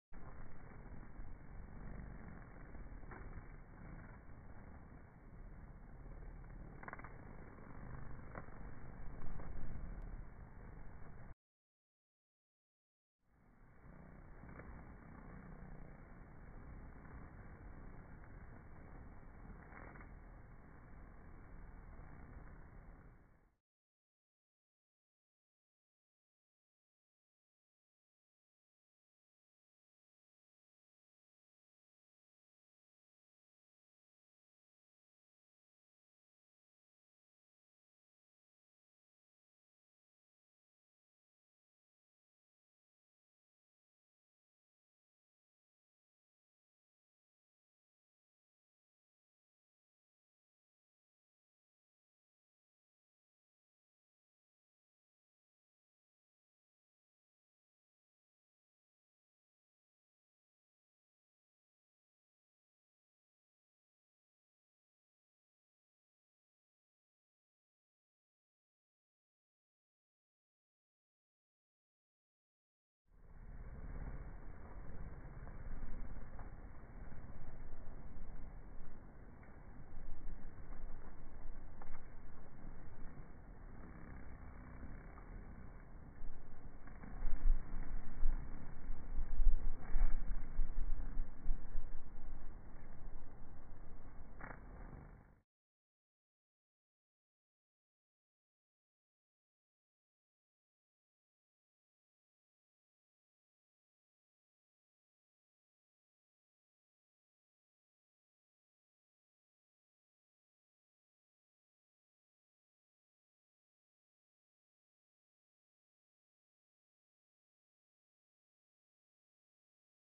Vanessa Atalanta Red Admiral butterfly Swarming Amazing slow motion
Vanessa Atalanta/Red admiral butterflies swarming in Tjörnarp, Sweden, 16 August 2019

Amiralfjärilar svärmar kring lockbete bestående av sockermättat rödvin i Kyrkbyn Tjörnarp, Skåne, 16 augusti 2019